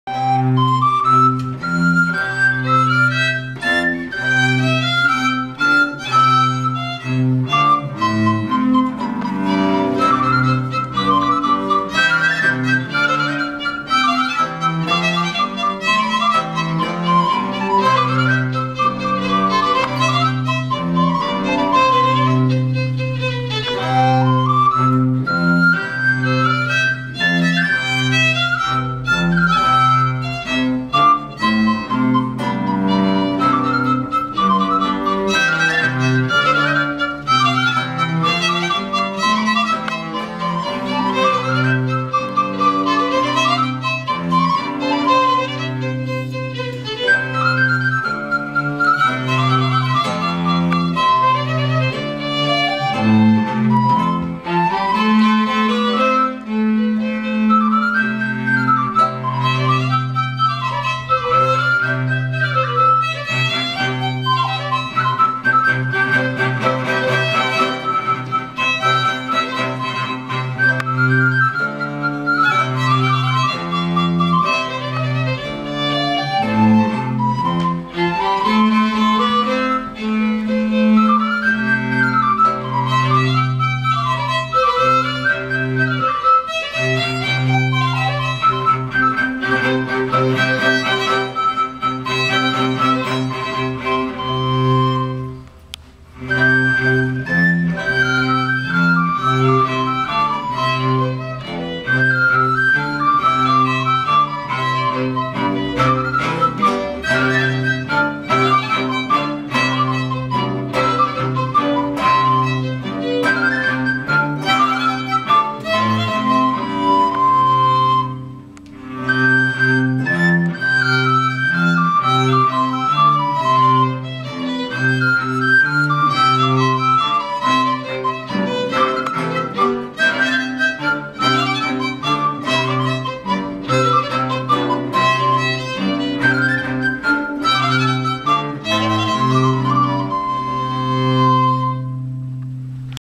Antoquo More, Łódź (9)
Koncert u Dminikanów w kamienicy na Zielonej